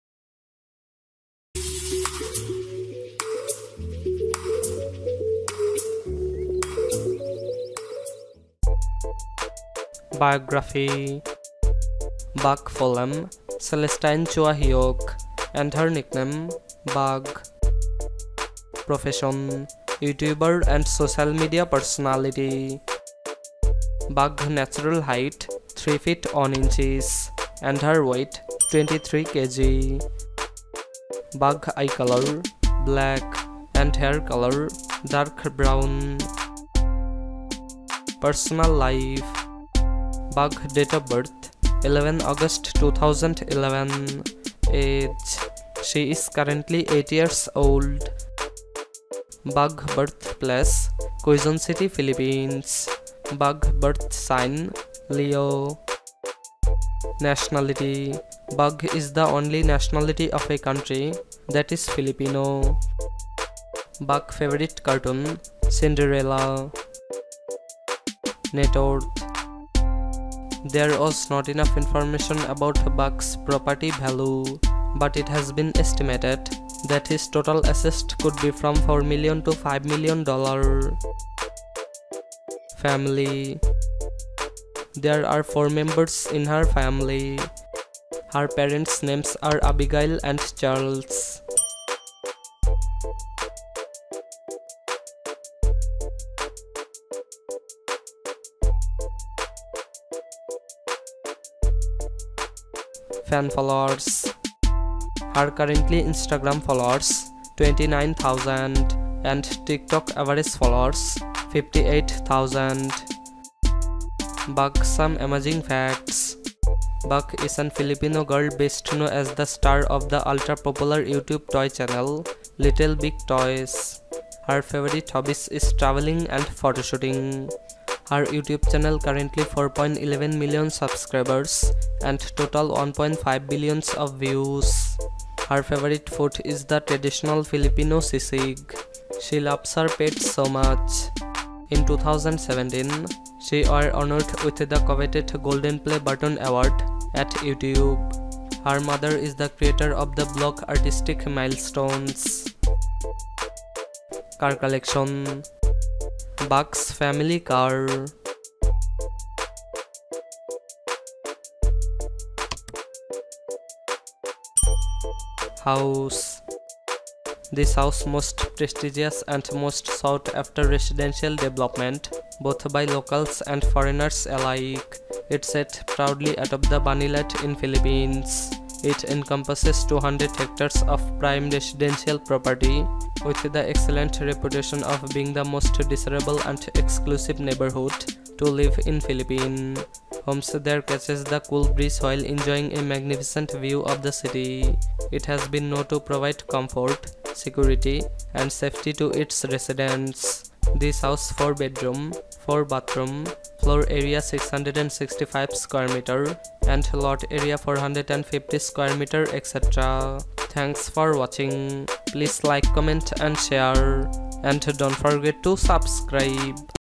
Little Big Toys (Bug) Biography,Net Worth,Boyfriend,Family,Cars,House & LifeStyle 2020
Bug is an Filipino girl best known as the star of the ultra-popular YouTube toy channel Little Big Toys.
Her favourite hobbies is traveling and photo shooting.
Her youtube channel currenty 4.11 million subscribers and total 1.5 billions of views.
Her favourite food is the traditional filipino Sisig.
She loves her pet so much.
In 2017, she were honoured with the coveted ‘Gold Play Button Award’ at YouTube.
Her mother is the creator of the blog Artistic Milestones.